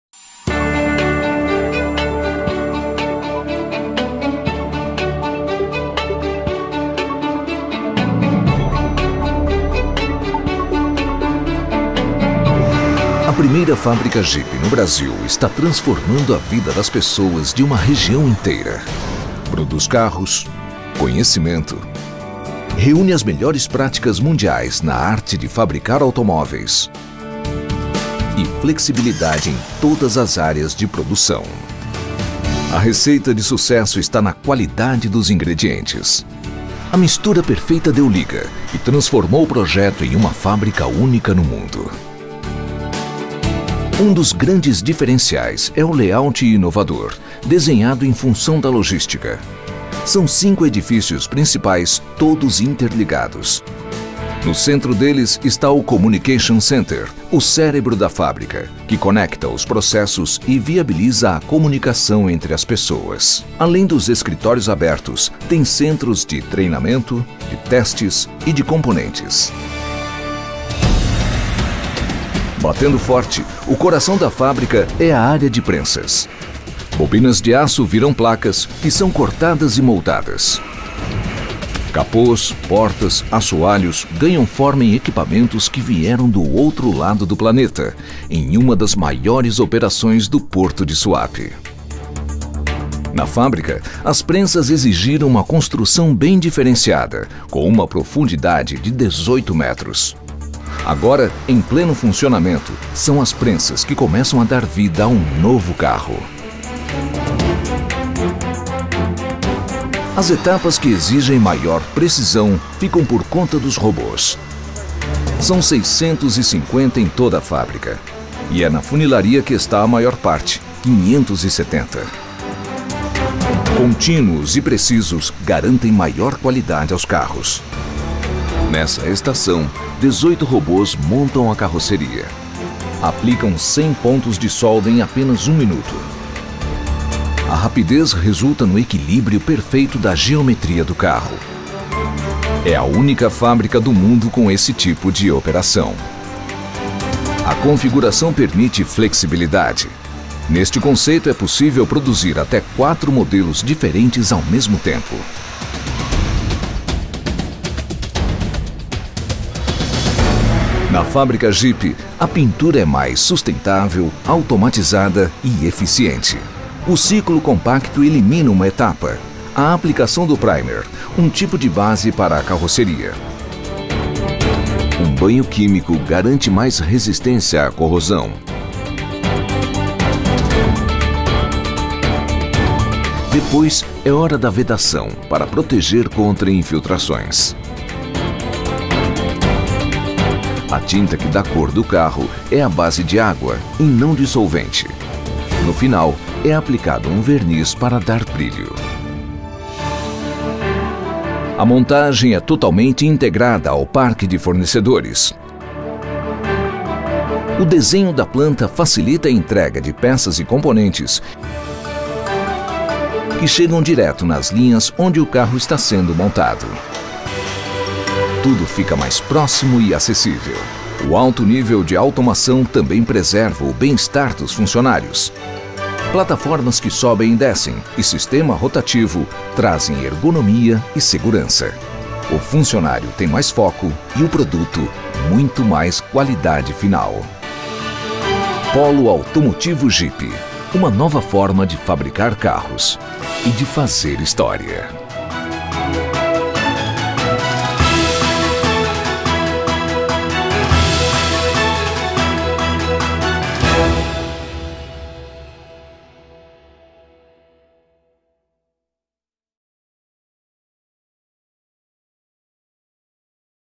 Polo Automotivo Jeep - A Operação do Complexo
O Polo Automotivo Jeep, com capacidade para produzir até 250 veículos por ano conta com a fábrica mais moderna da FCA no mundo.

Além de um design inovador da planta Jeep, o polo conta com cerca de 9.000 funcionários e vai abastecer toda a América Latina